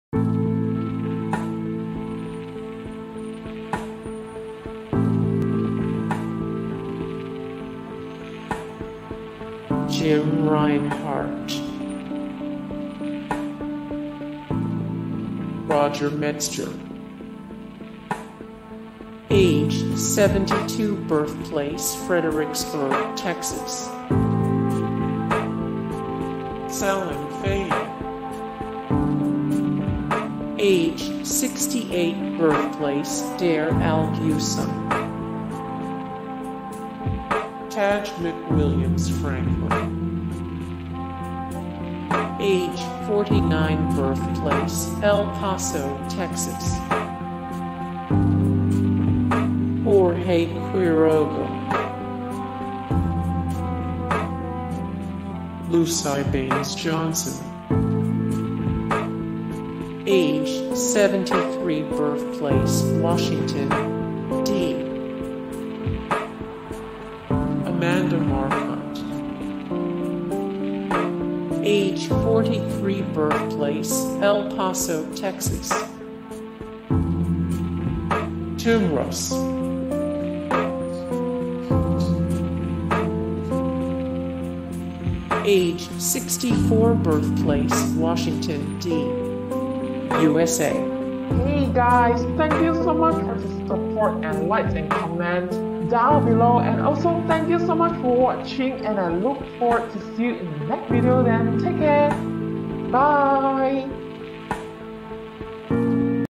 Famous St. Edward'S University Alumni ➊
List of famous alumni from St. Edward's University, with photos when available. Prominent graduates from St. Edward's University include celebrities, politicians, business people, athletes and more. This list of distinguished St. Edward's University alumni is loosely ordered by relevance, so the most recognizable celebrities who attended St. Edward's University are at the top of the list. This directory is not just composed of graduates of this school, as some of the famous people on this list didn't necessarily earn a degree from St. Edward's University.  Examples include Tim Russ and Jorge Quiroga. This list answers the questions Which famous people went to St. Edward's University? and Which celebrities are St. Edward's University alumni?...more
0:00 - Intro
0:00:08 - Jim Reinhart
0:00:15 - Roger Metzger
0:00:27 - Salam Fayyad
0:00:39 - Taj McWilliams-Franklin
0:00:52 - Jorge Quiroga
0:00:59 - Luci Baines Johnson
0:01:12 - Amanda Marcotte
0:01:24 - Tim Russ